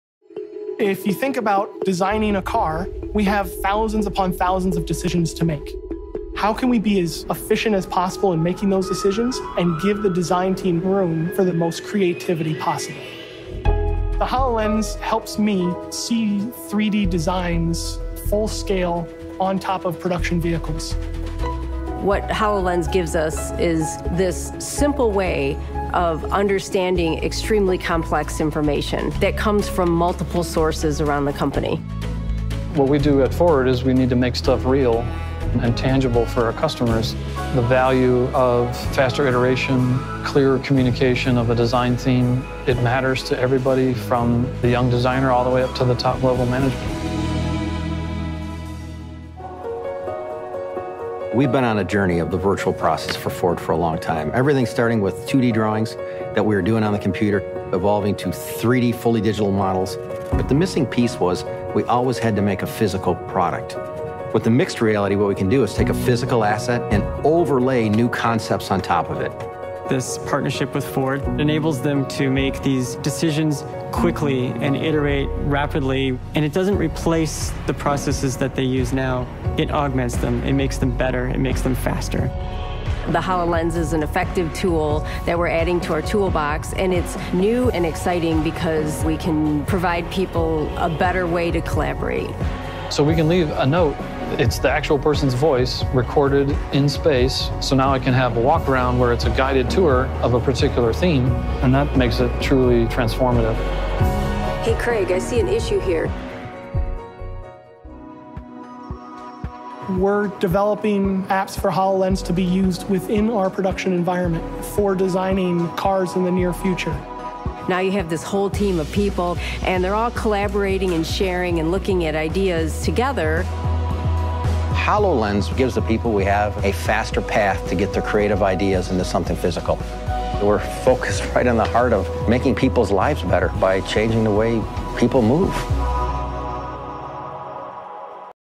Car Designers at Ford Using Holographic Technology
See how car designers at Ford are using Microsoft HoloLens mixed reality technology. Full story